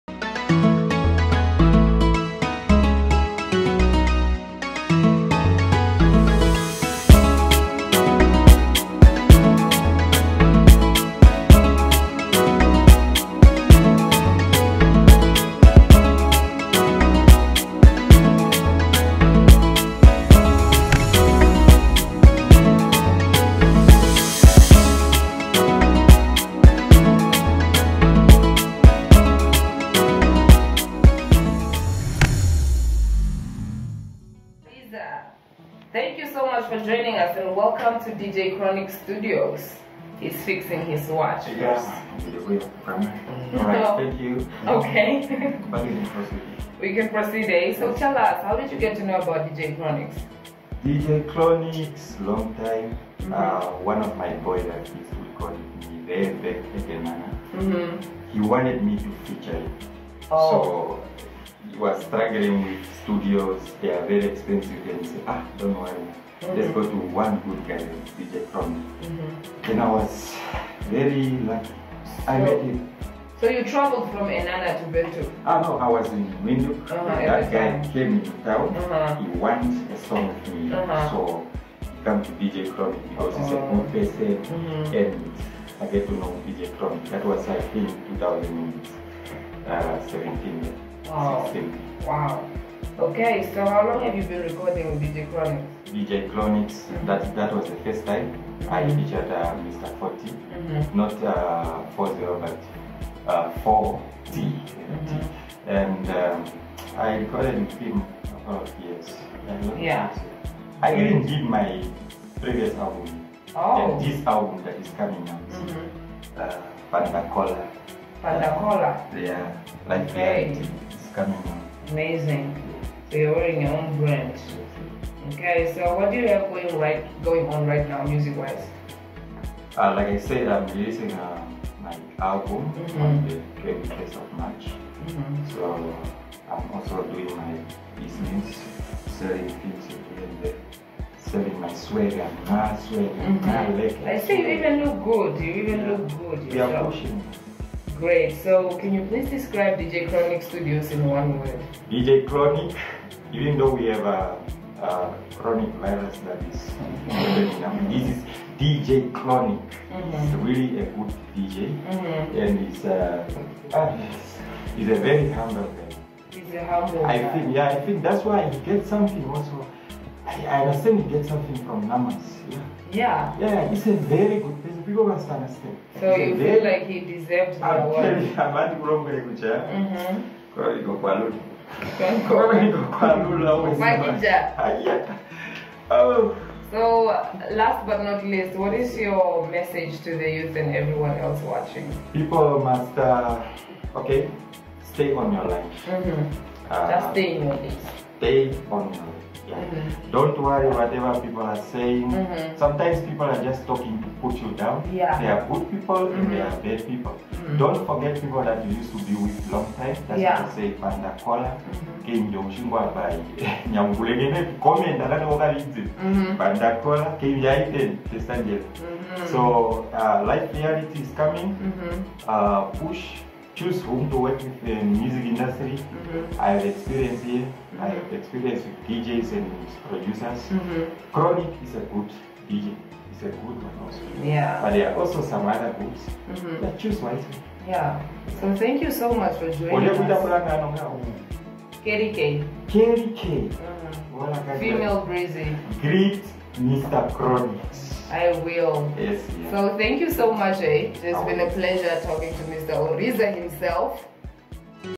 Chat With Orizza || Namibian Artist || 2020
Episode 02

Interviewed By Carry_K
Beat / Music By Dj Chronic Official

Dj chronic Studio

ABOUT DJ CHRONIC 👇👇👇
DJ Chronic is a renowned Namibian producer. In 2019 DJ Chronic was nominated and eventually won the Best Producer's award at the NAMAs.